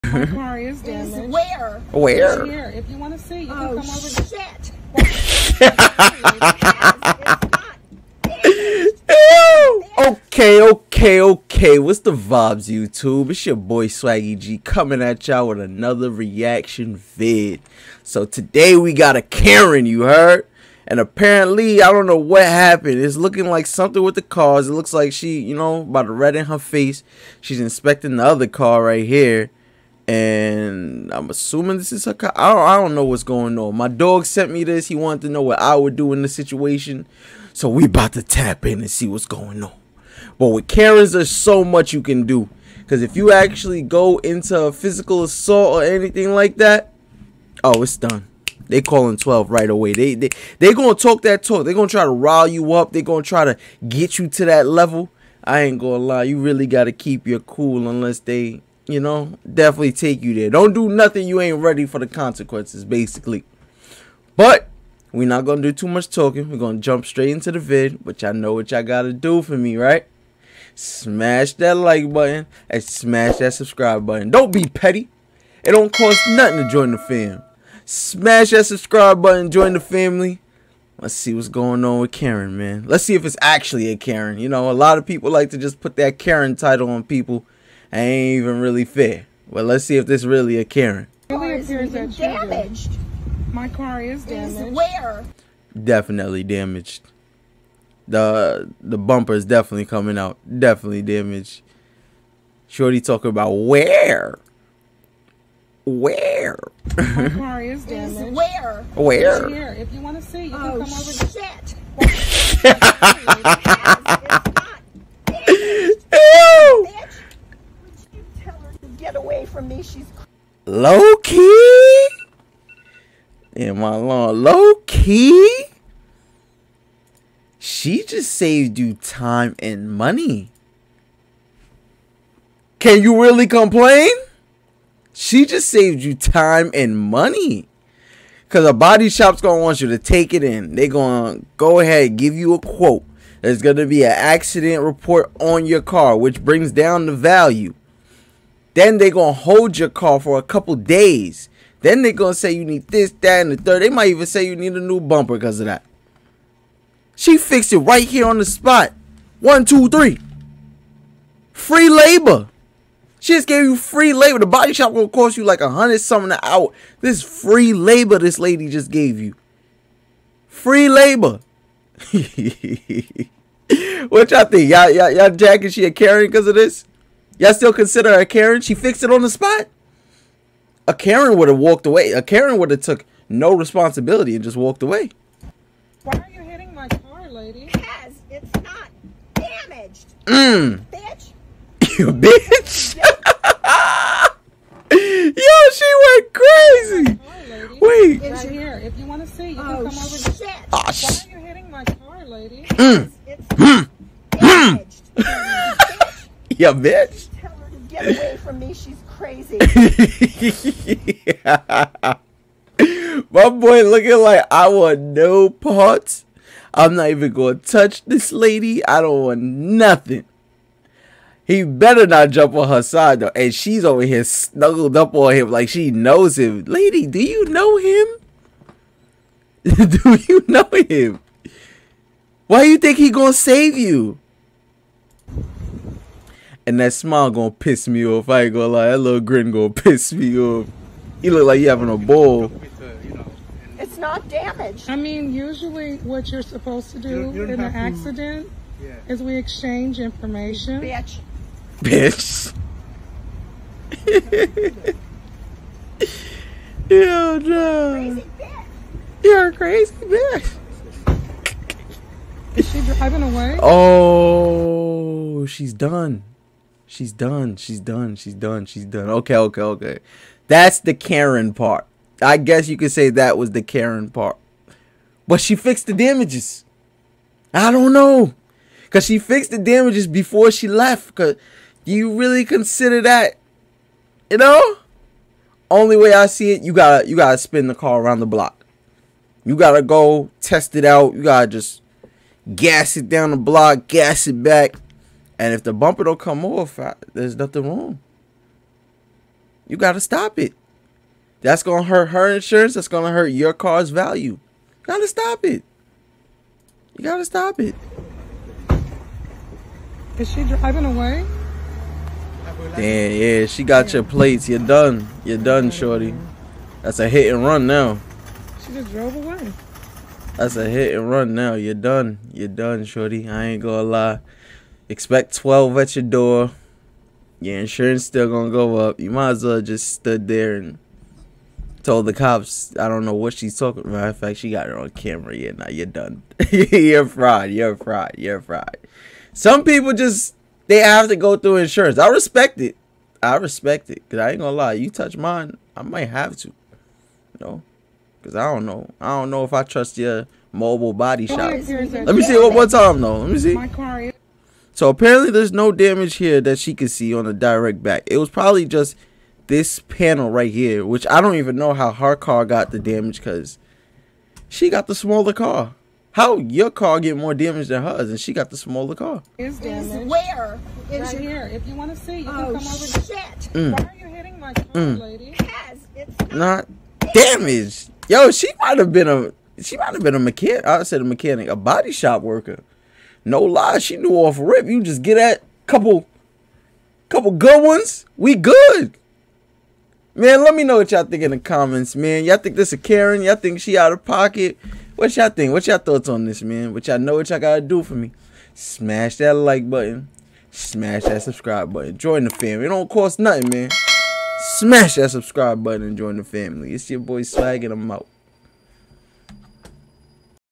SWAGGYTV REACTS TO COSTCO Karen HAS Tantrum Against Black Woman After Fender Bender COPS GOT CALLED
SWAGGYTV REACTS TO Costco Karen Unleashes BIZARRE Tantrum Against Black Woman After Fender Bender, Drives Away

Thank you for tunings in your comments, and support is much appreciated.

I do not take any ownership of music displayed in this video. Ownership belongs to the respected owner(s).